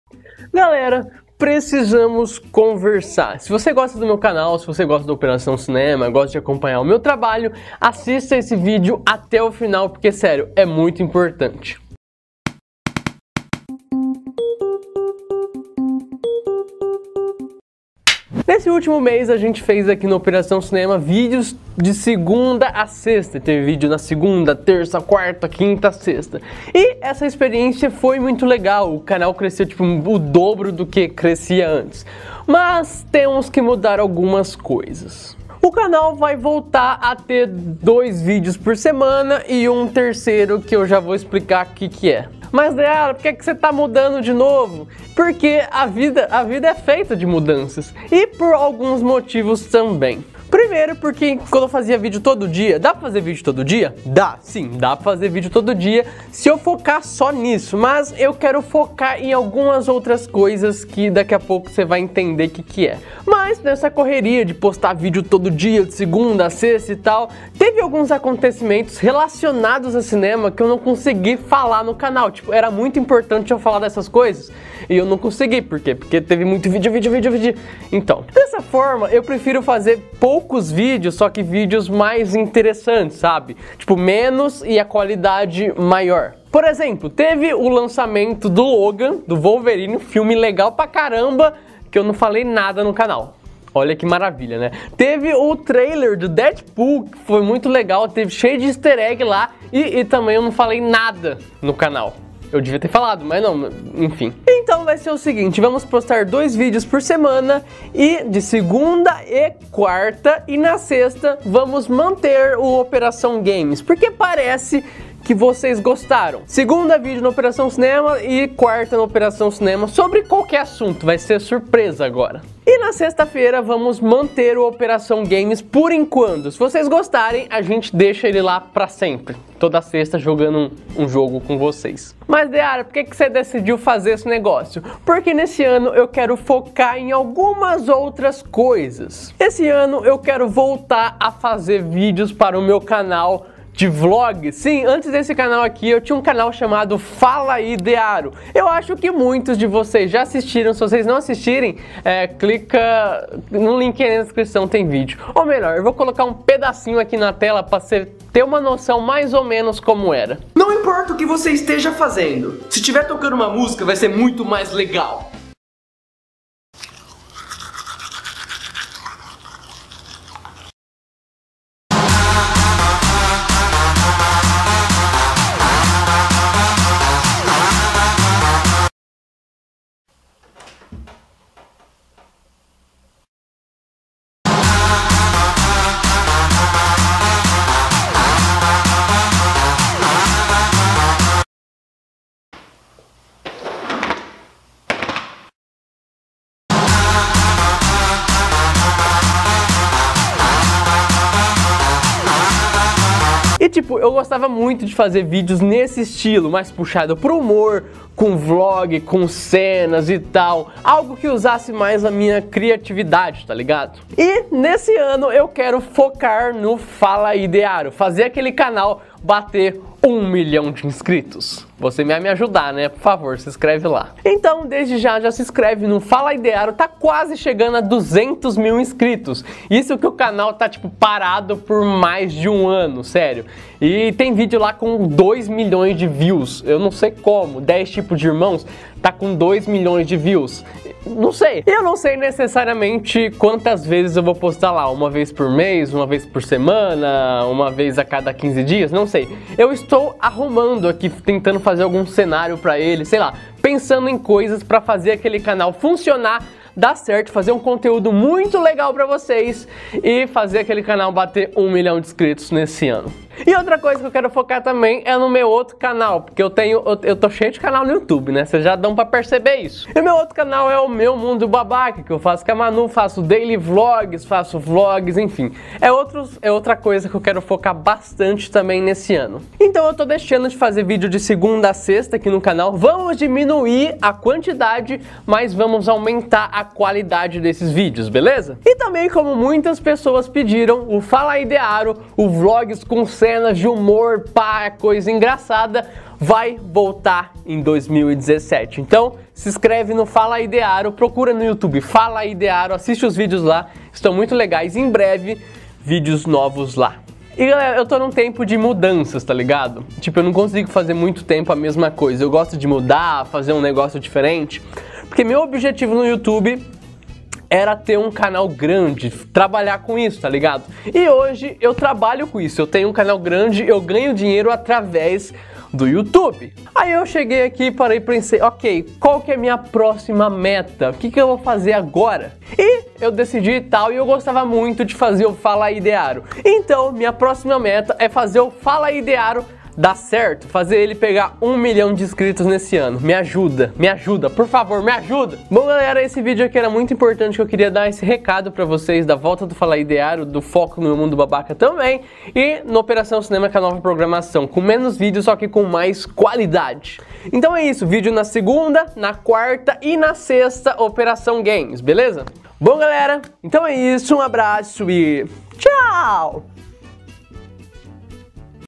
PRECISAMOS CONVERSAR!
Nesse vídeo falo sobre algumas mudanças do canal Operação Cinema e a volta do incrível Falaidearo!

➡ Vídeos novos de Segunda a Sexta às 19 Horas!